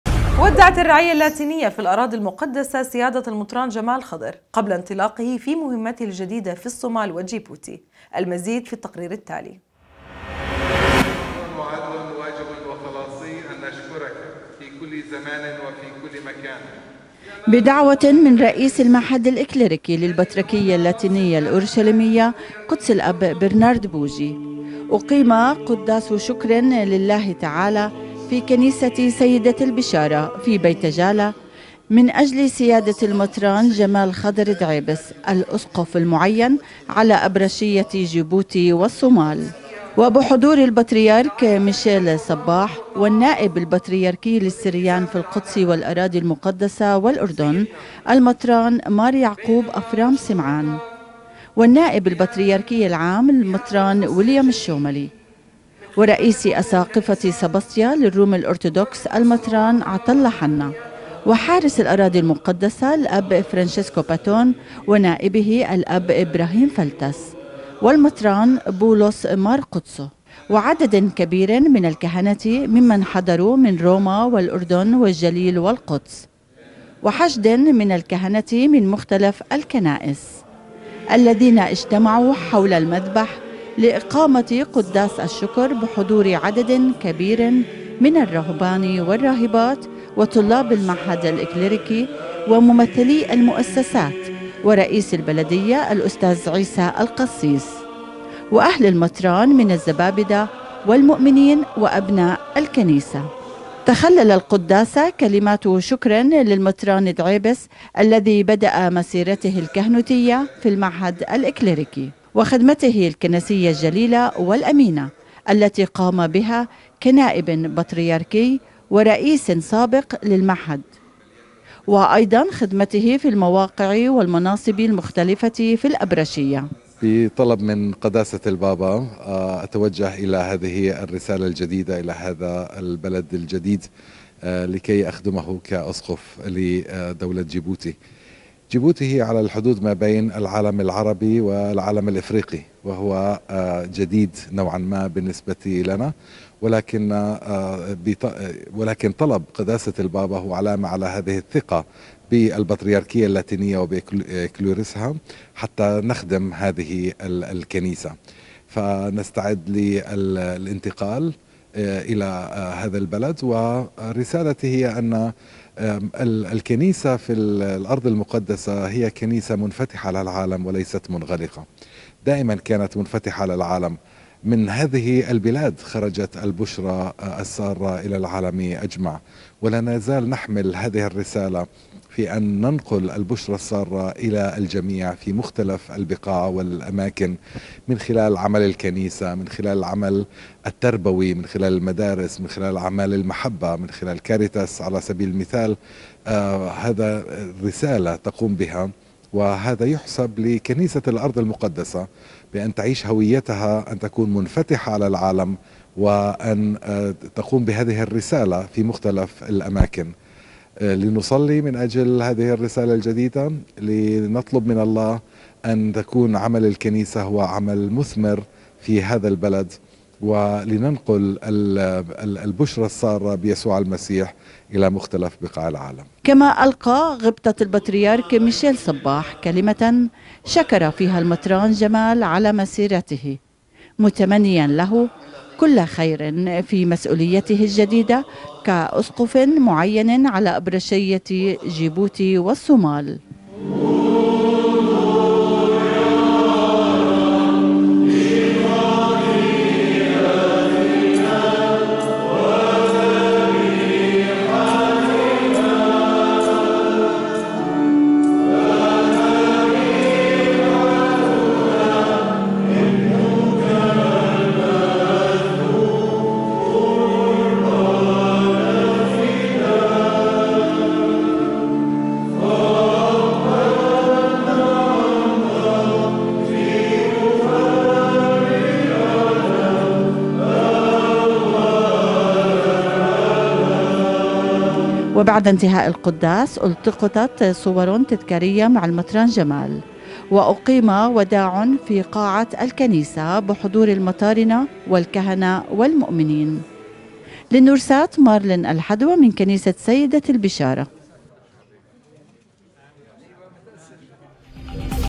ودّعت الرعيةُ اللاتينيةُ في الأرضِ المقدسه سيادةَ المطران جمال خضر
ودّعت الرعيةُ اللاتينيةُ في الأرضِ المقدسه سيادةَ المطران جمال خضر قبل إنطلاقهِ في مهمتهِ الجديدةِ كأسقفٍ على أبرشيةِ جيبوتي والصومال / وذلك في كنيسةِ البشاره في بيت جالا بحضور عددٍ من الكهنةِ والراهباتِ وجمعٍ كبيرٍ من المؤمنينَ القادمينَ من فلسطينَ والأردنِ وروما / المزيد في رسالة الأرض المقدسه